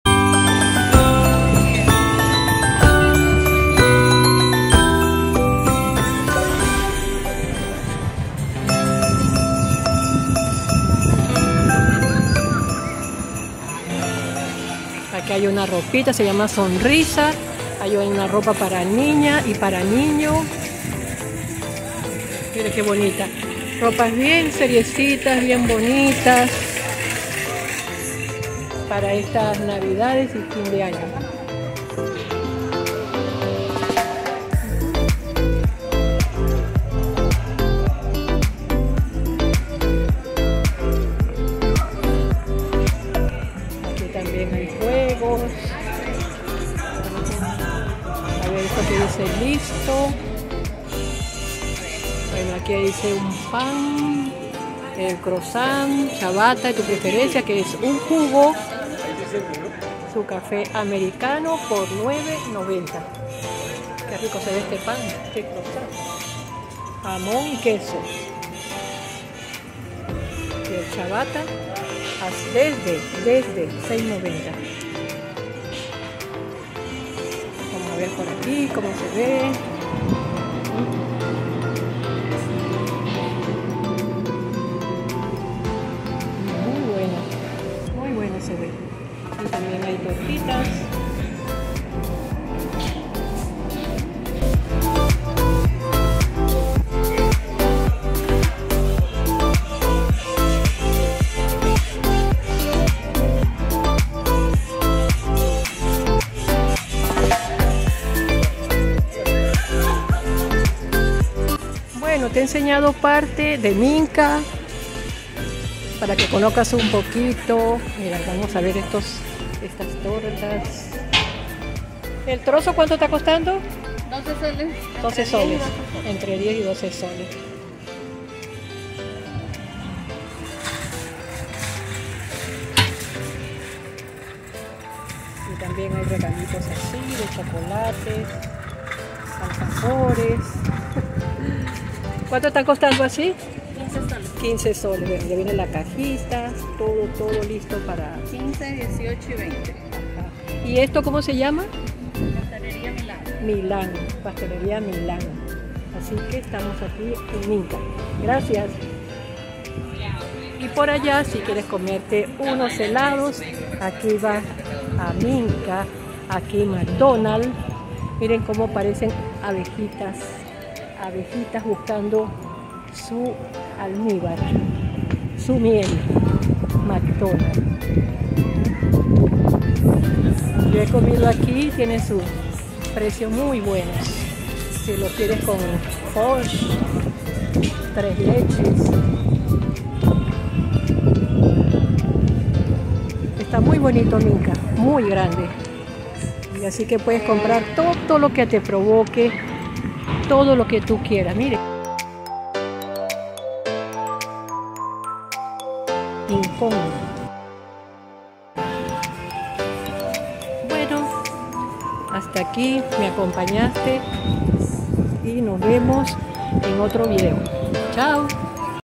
🎄🎁🤪EL CENTRO COMERCIAL MÁS GRANDE DE TODO PERÚ.
Recorriendo estos lugares de este centro comercial llamado Minka que hay muchos limeños que todavía no lo conocen y pueden conseguir a mejores precios y muy económicos.

Se encuentra en La avenida Argentina, cerca de Fauccet.